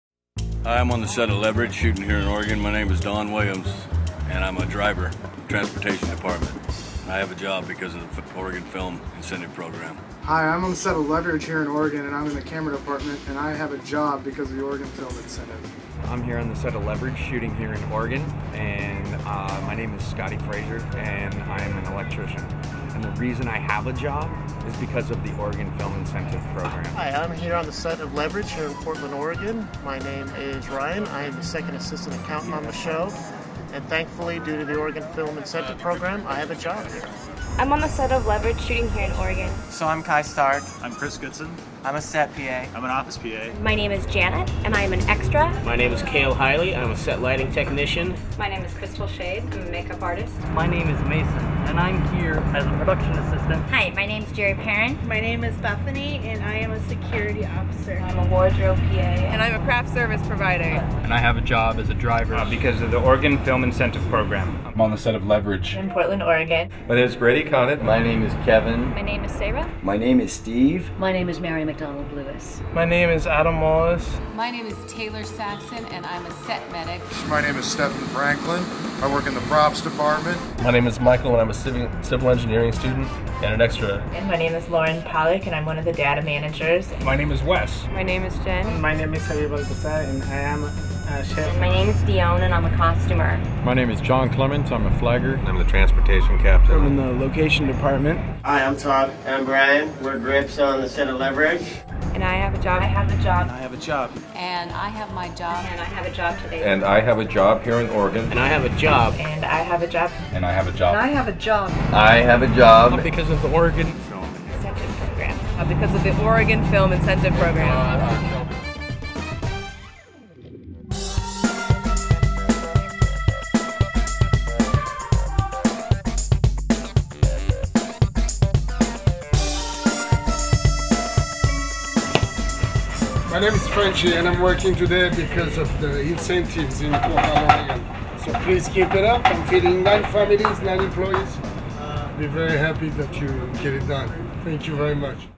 A special thanks
A sincere thank you to Governor John Kitzhaber and the Oregon Legislators for supporting the Oregon Film Incentive and creating jobs.